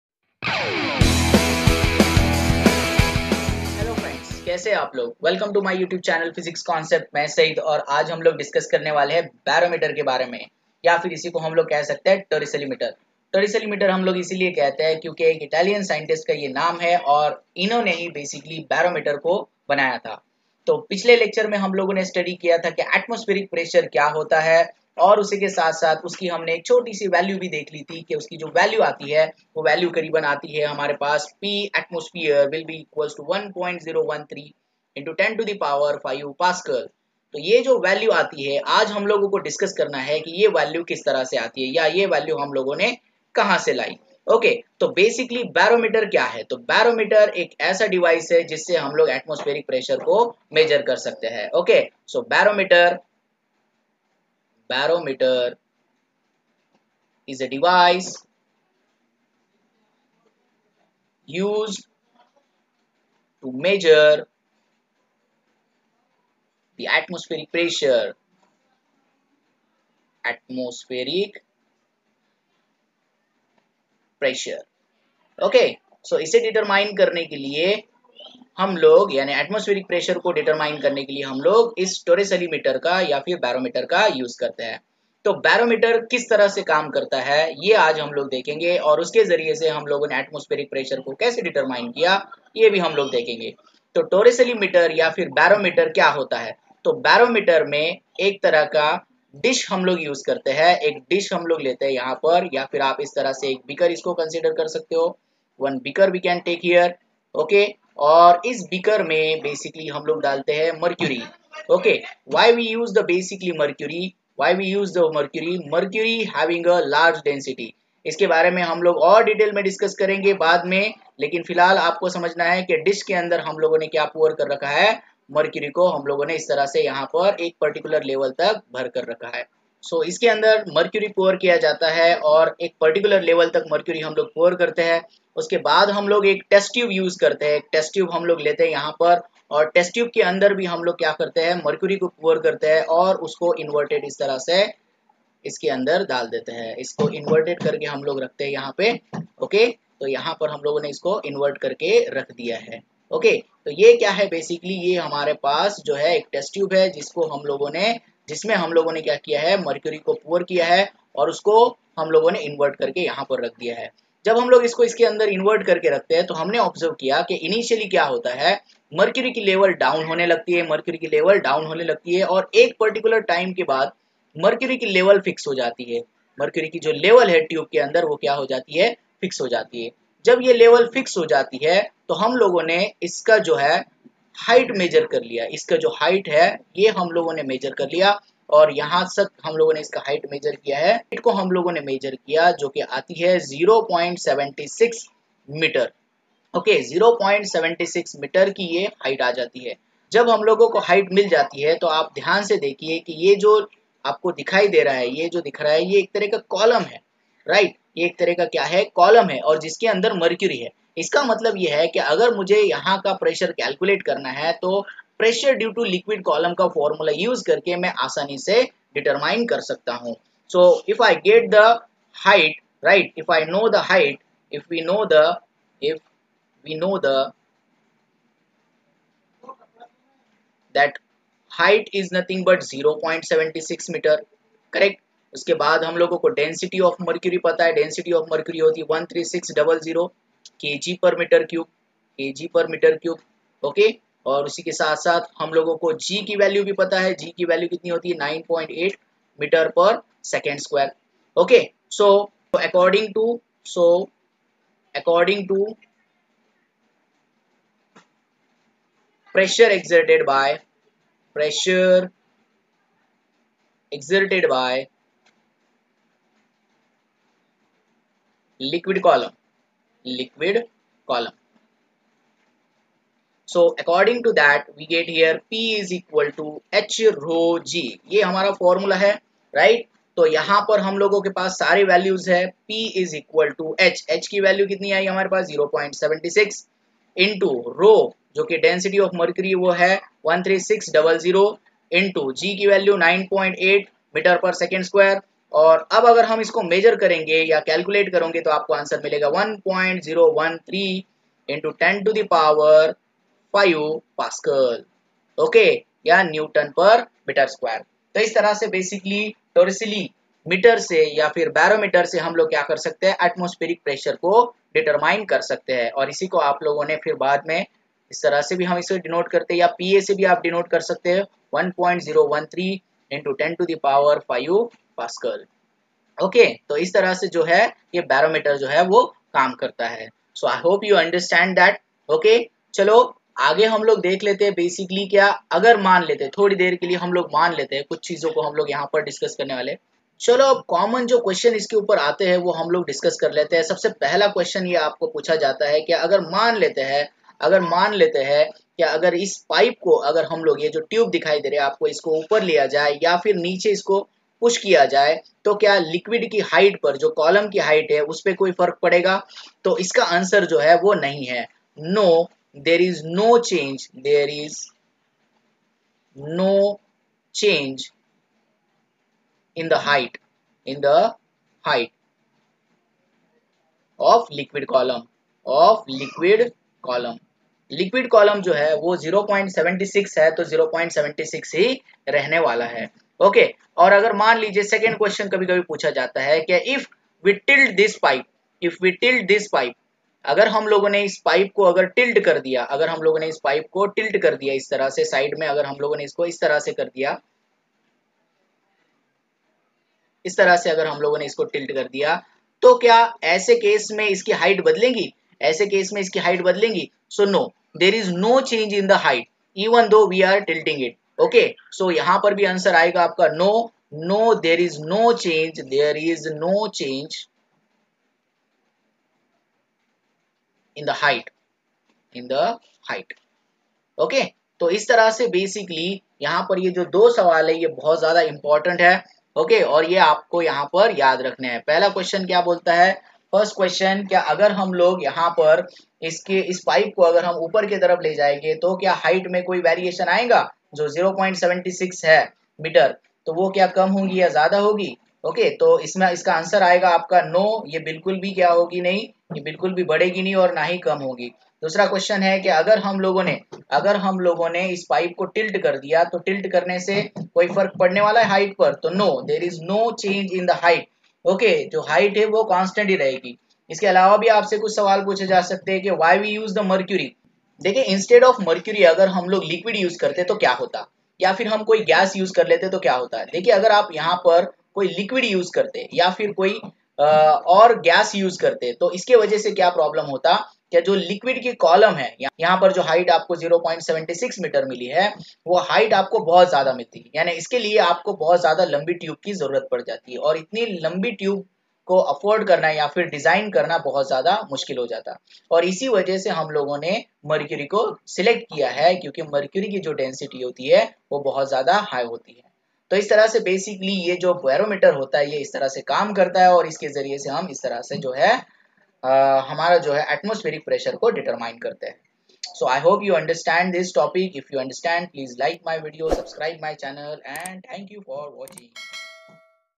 Barometer ( measurement of atmospheric pressure)
Hello friends
I am Saeed Shaikh and here is the video on barometer
In this video we understand what is meant by barometer why it is also called Torricelli metre
And how to use barometer to calculate atmospheric pressure
This video is on barometer which is the instrument or device used to determine atmospheric pressure
Barometer is first device used to determine atmospheric pressure

So please watch this video you will definitely understand barometer and it's use